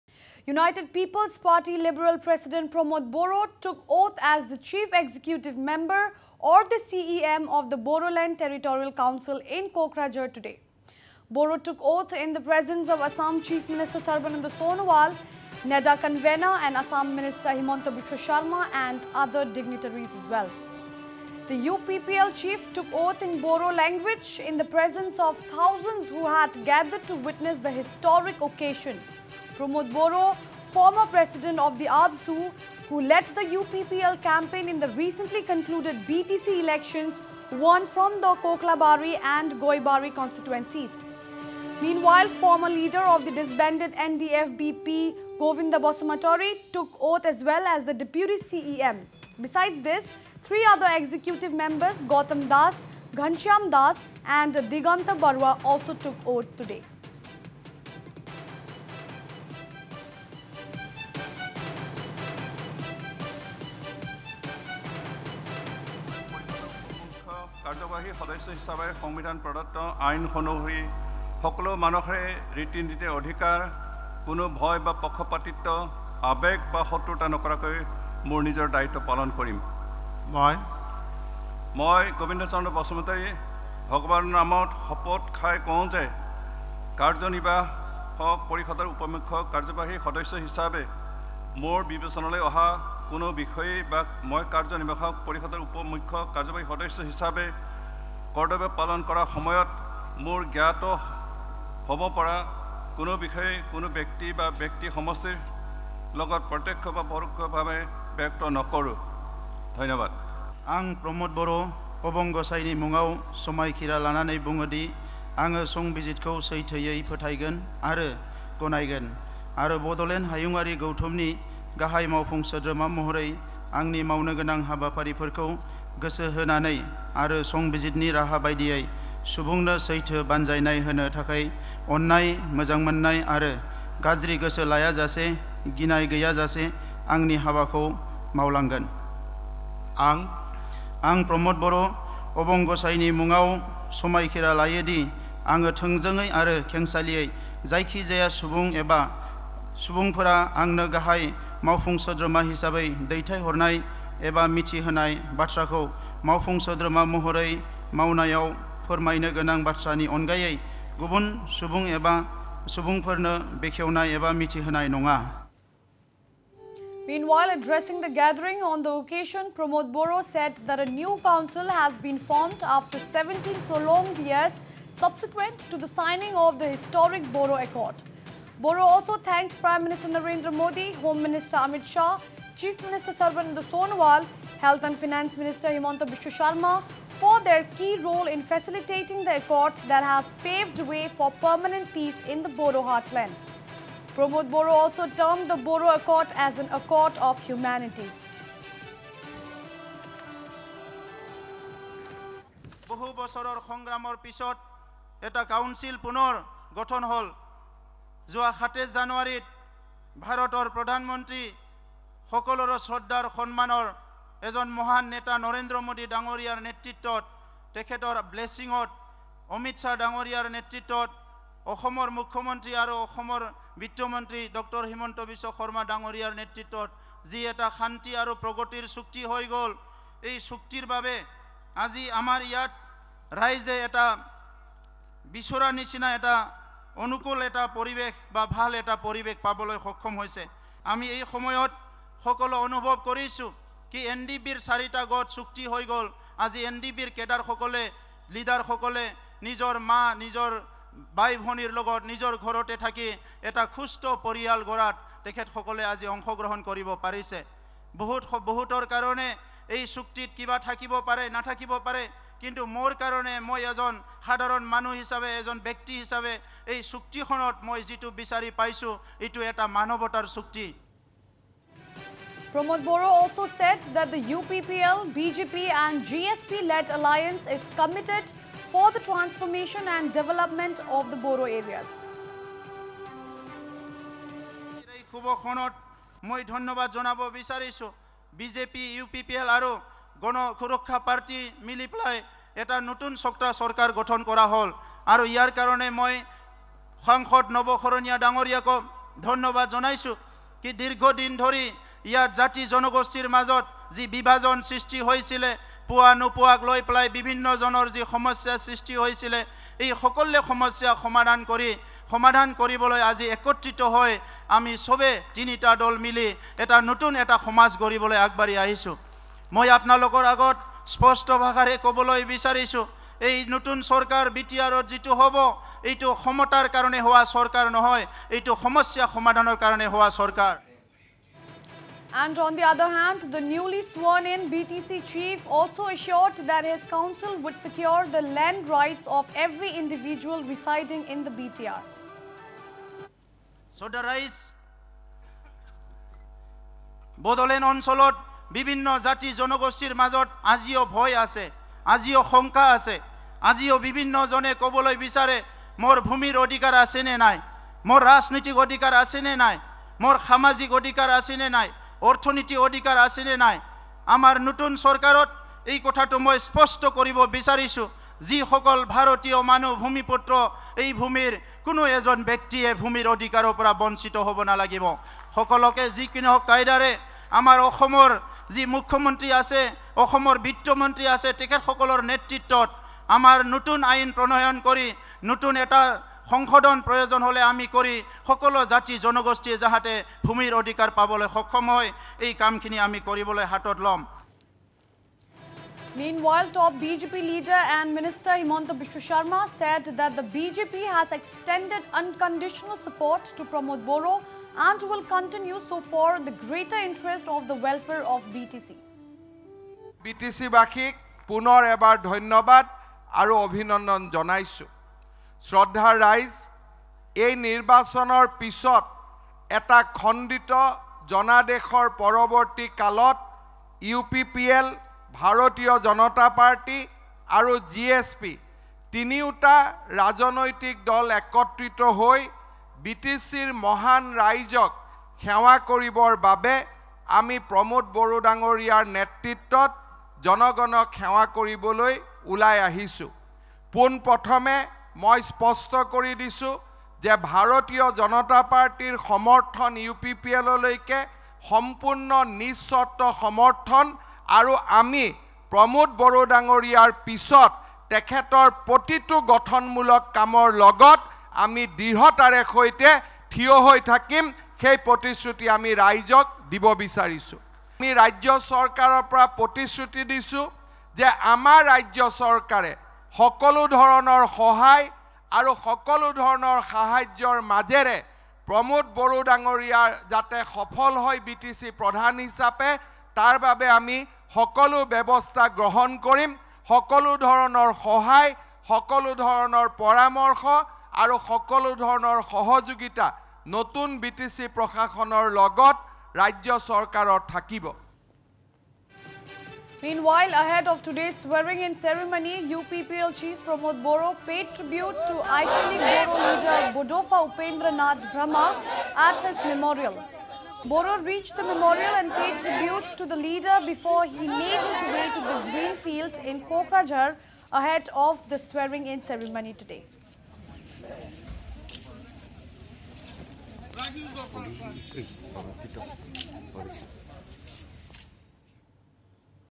Pramod Boro sworn in as the new CEM of the Bodoland Territorial Council
United People's Party Liberal President Pramod Boro took oath as the Chief Executive Member or the CEM of the Bodoland Territorial Council in Kokrajhar today.
Boro took oath in the presence of Assam Chief Minister Sarbananda Sonowal, NEDA convener and Assam Minister Himanta Biswa Sarma and other dignataries.
The UPPL chief took oath in Boro language in the presence of thousands who had gathered to witness the historic occasion.